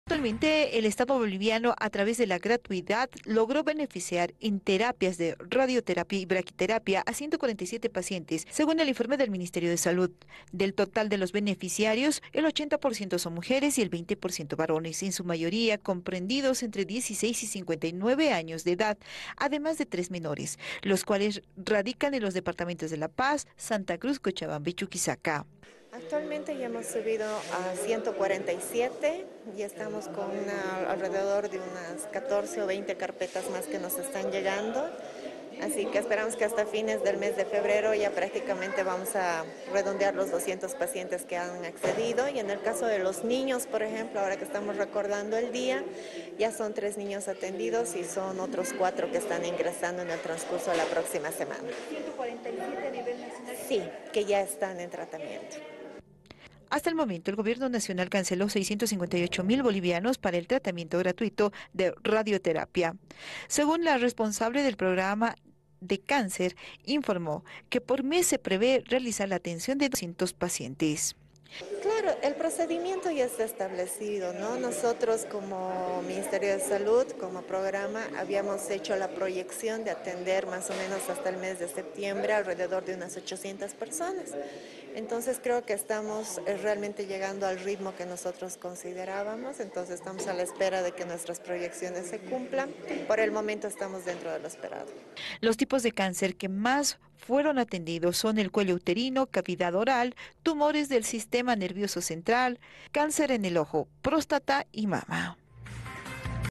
El Estado atendió gratuitamente a 147 pacientes de cáncer adultos y tres niños
Abya Yala Digital.- En tres meses, 147 pacientes con cáncer de los departamentos de La Paz, Santa Cruz, Cochabamba y Chuquisaca, recibieron tratamiento gratuito en hospitales privados, además tres niños son beneficiados con este tratamiento. Del total de beneficiarios, el 80% son mujeres y 20% varones, en su mayoría comprendidos entre 16 y 59 años. Según Sdenka Maury Fernández, responsable del Programa Cáncer, señaló que se espera terminar febrero redondeando los 200 pacientes atendidos, y también dio a conocer que 4 niños se encuentra en carpeta para que ser atendidos con el tratamiento